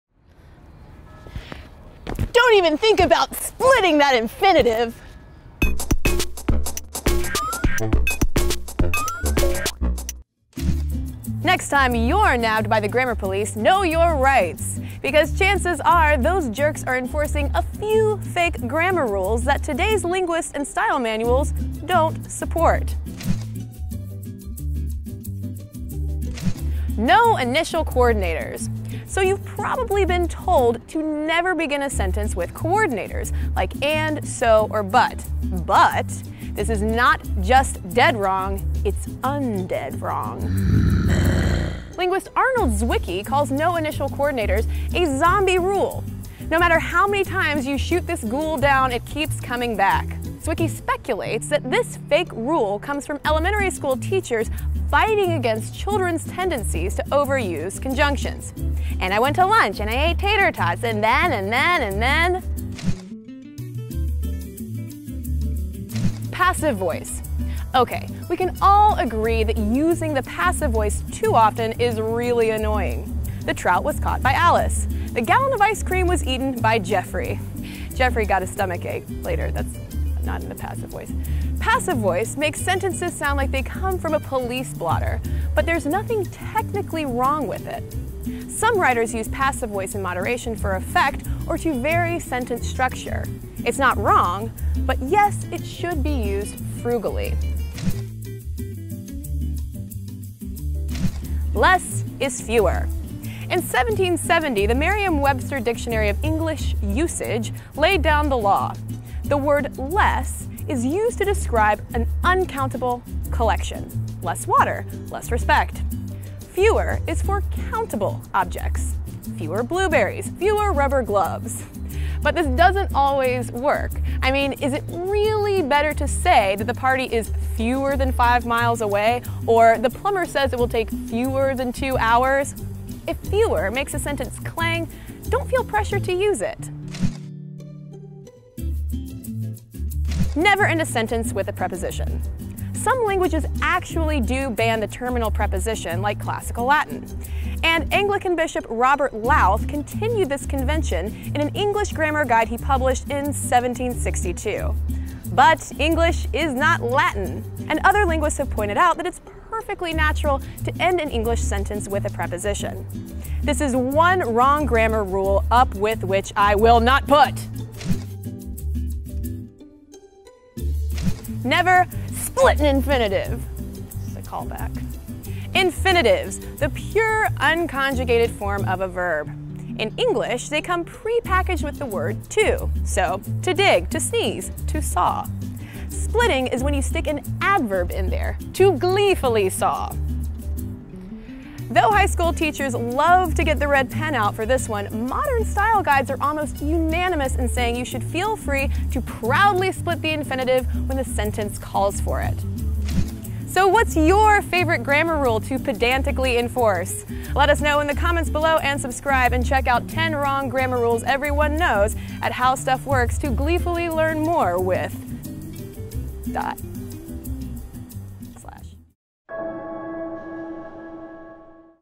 5 Wrong Grammar Rules Everyone Knows | What the Stuff?!
Next time you’re nabbed by the grammar police, know your rights! Because chances are those jerks are enforcing a few fake grammar rules that today’s linguists and style manuals don’t support.

What the Stuff?! episodes are available every Monday and Friday at Noon ET.

No Initial Coordinators! So you’ve probably been told never to begin a sentence with “coordinators” like “and,” “so” or “but.” But this is not just dead wrong; it’s un-dead wrong. Linguist Arnold Zwicky calls “No Initial Coordinators” a “zombie rule”; no matter how many times you shoot this ghoul down, it keeps coming back. Zwicky speculates that the fake rule comes from elementary school teachers fighting against children’s tendency to overuse conjunctions: “And I went to lunch. And I ate tater tots. And then, and then, and then…”

Passive voice. OK, we can all agree that using the passive voice too often is really annoying. “The trout was caught by Alice.” “The gallon of ice cream was eaten by Jeffrey.” Passive voice makes sentences sound like they come from a police blotter, but there’s nothing technically wrong with it. Some writers use passive voice in moderation for effect or to vary sentence structure. It’s not wrong. But yes, it should be used frugally.

Less is fewer. In 1770, the Merriam-Webster Dictionary of English Usage laid down the law: The word “less” is used to describe an uncountable collection: “less water,” “less respect.” “Fewer” is for countable objects: “fewer blueberries,” “fewer rubber gloves.” But this doesn’t always work. I mean, is it really better to say “The party is fewer than 5 miles away,” or “The plumber says it will take fewer than 2 hours?” If “fewer” makes the sentence clang, don’t feel pressure to use it.

Never end a sentence with a preposition. Some languages actually do ban the terminal preposition, like Classical Latin. And the Anglican bishop Robert Lowth continued this convention in an English grammar guide he published in 1762. But English isn’t Latin, and other linguists have pointed out that it’s perfectly natural to end an English sentence with a preposition. This is one wrong grammar rule up with which I will not put.

Never split an infinitive. Infinitives: The pure, un-conjugated form of a verb. In English, they come pre-packaged with the word “to.” So: “To dig,” “to sneeze,” “to saw”. Splitting is when you stick an adverb in there: “to gleefully saw.” Though high school teachers love to get the red pen out for this one, modern style guides are almost unanimous in saying you should feel free to proudly split infinitives when the sentence calls for it.

What’s your favorite grammar rule to pedantically enforce? And check out 10 Wrong Grammar Rules Everyone Knows at HowStuffWorks to learn more!